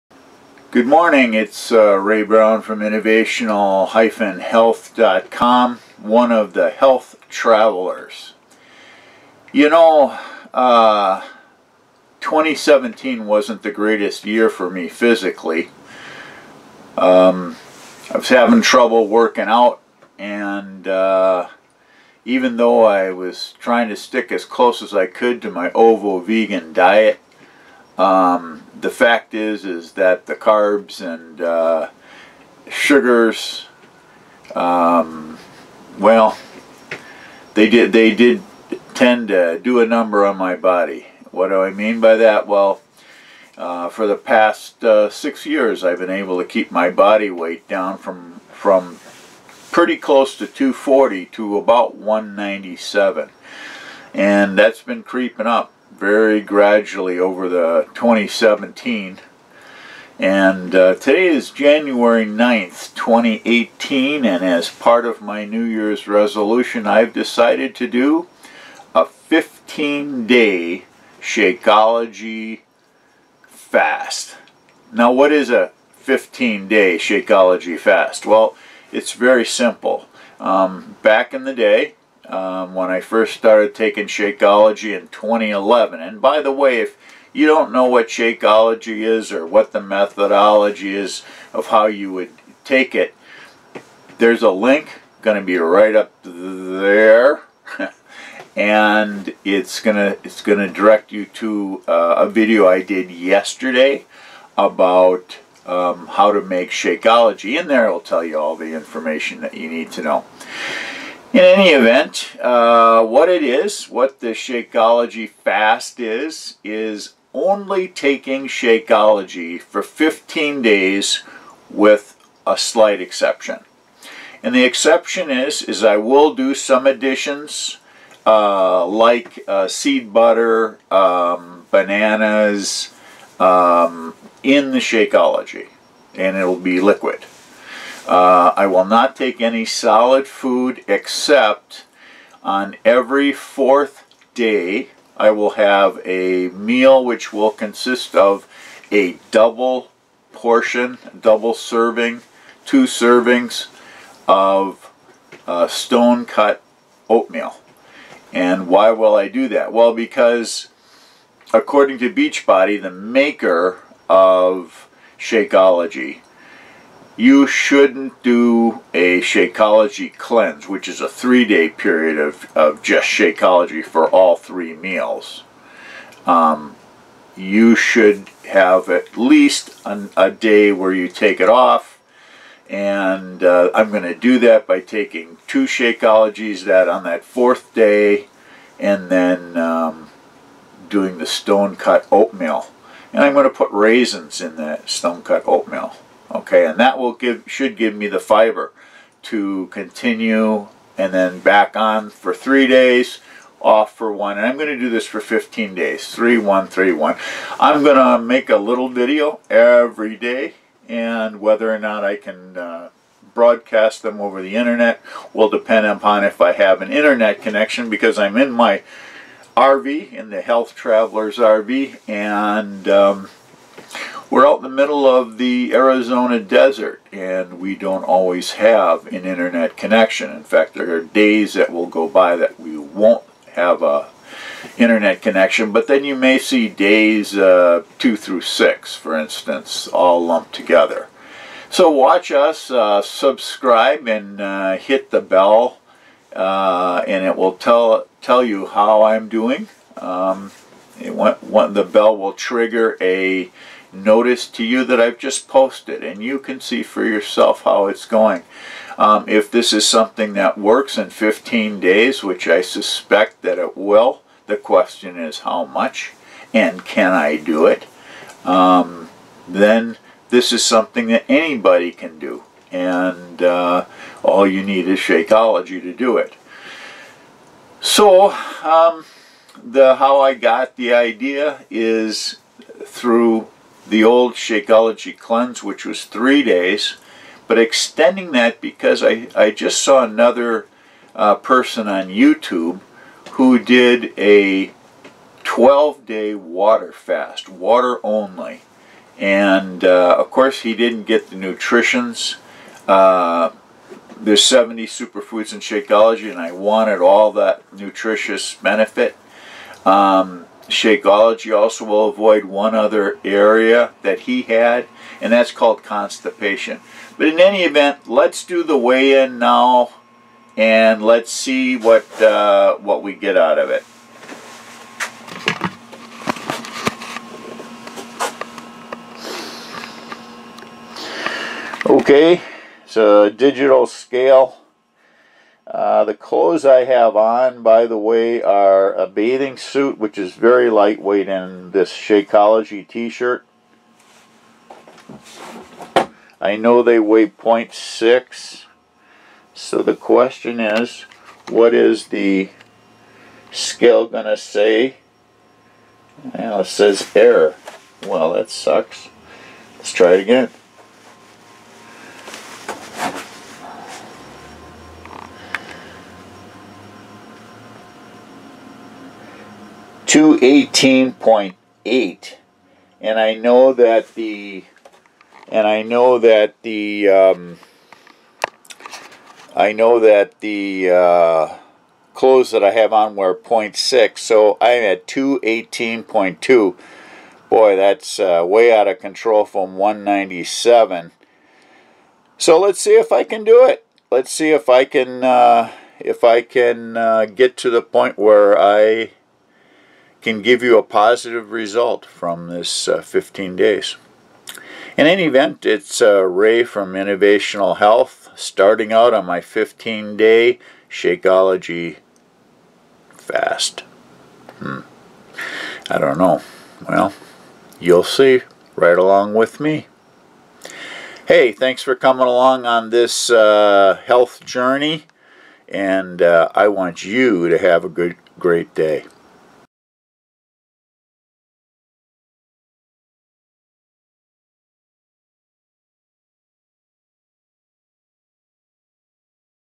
Starting a 15-Day Shakeology Fast
Need to lose weight in a couple of weeks? Don't want to just to do a water-only fast because you do want to be as healthy as possible? Try the 15-Day Shakeology Fast!

What is Shakeology?

Presented by Health Travellers.